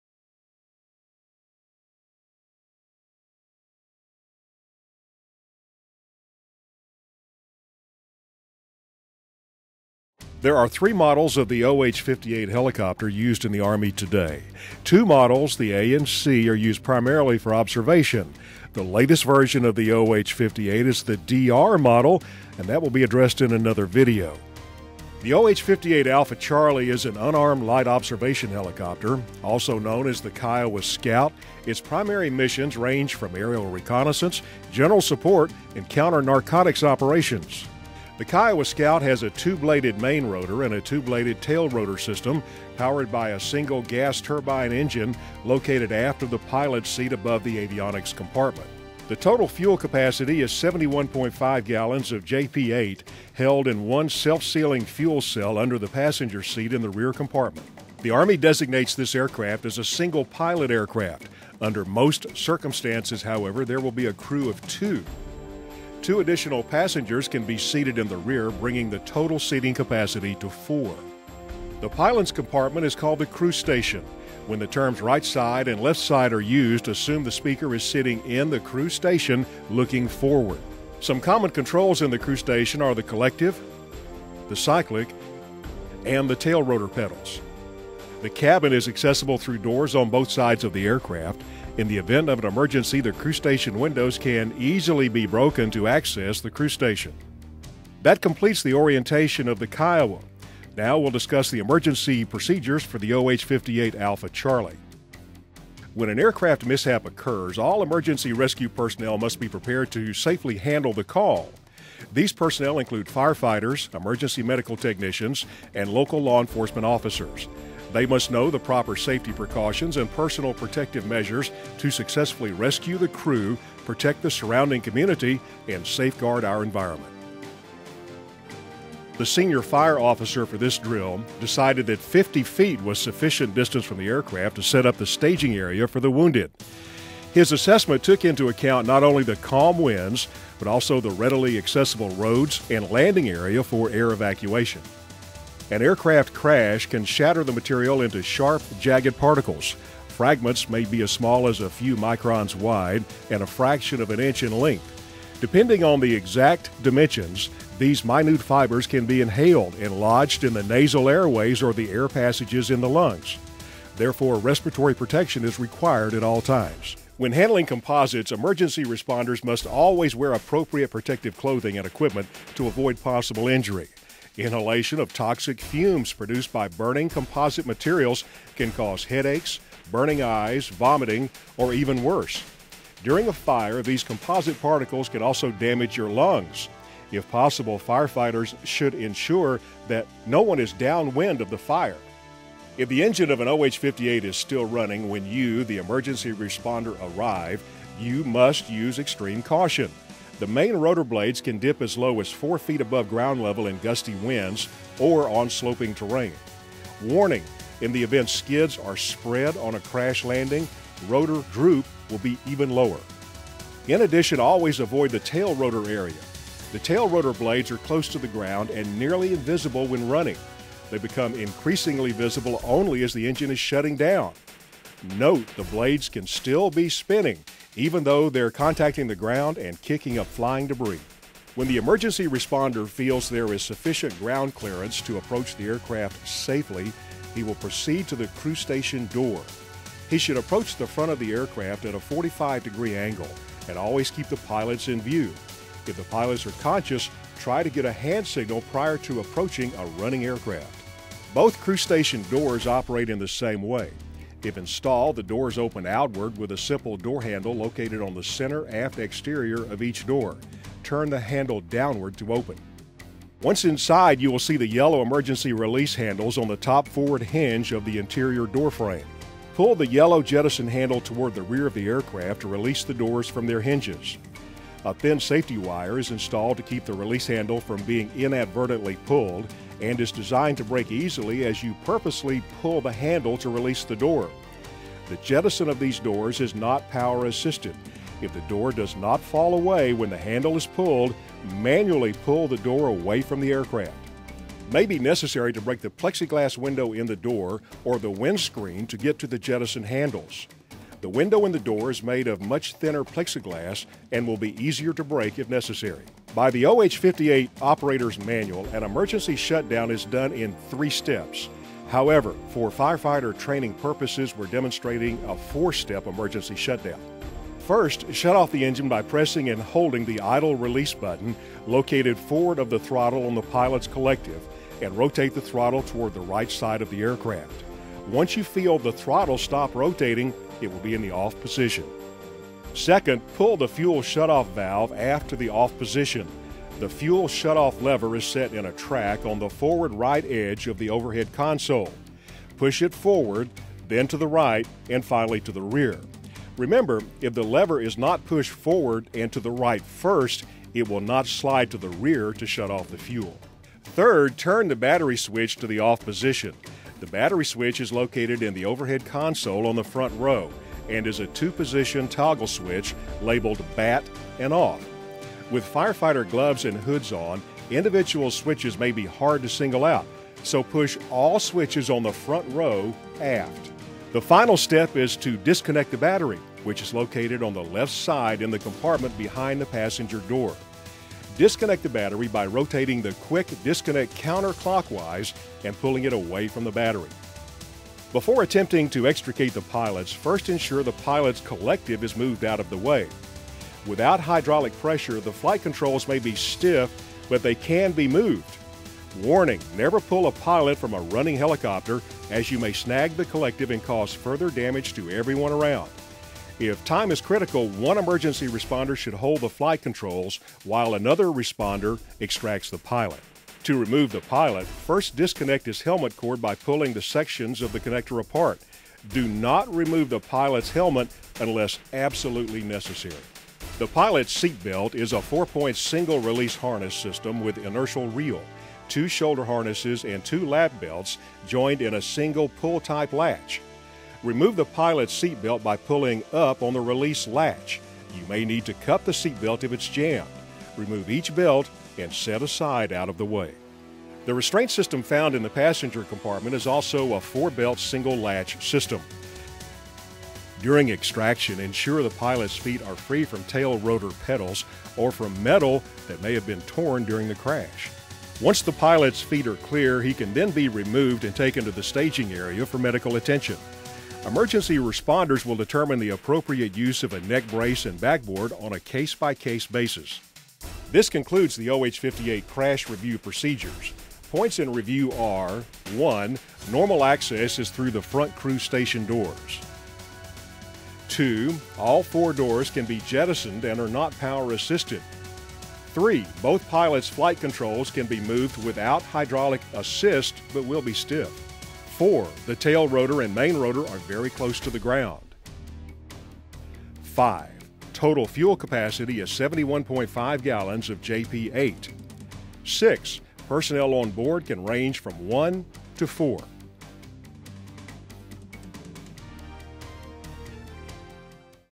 Crash/rescue video for the OH-58A/C helicopter
Here's the latest video from the U.S. Army Combat Readiness/Safety Center at Fort Rucker, Ala., on crash rescue procedures for the OH-58A/C helicopter. Army safe is Army strong!